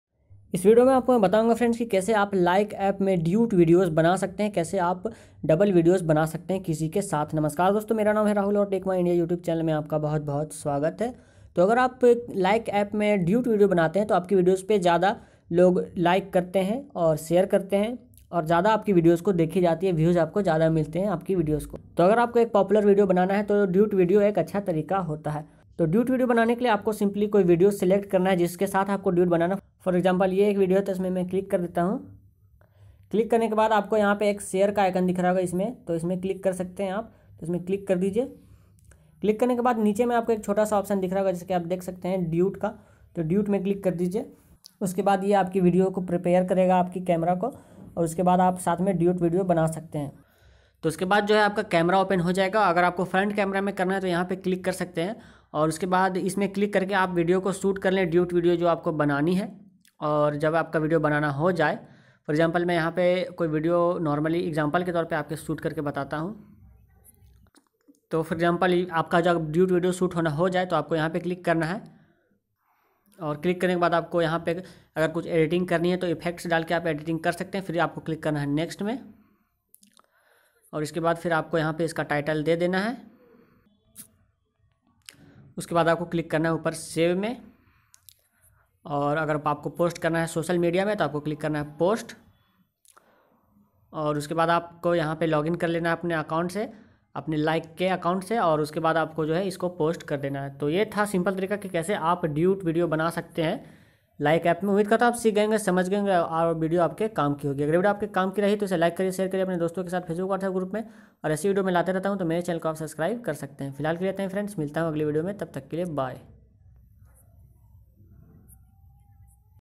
Like app me duet video kaise banaye | How to create duet video on like app
In this video i gonna show you Like app me duet video kaise banaye | How to create duet video on like app.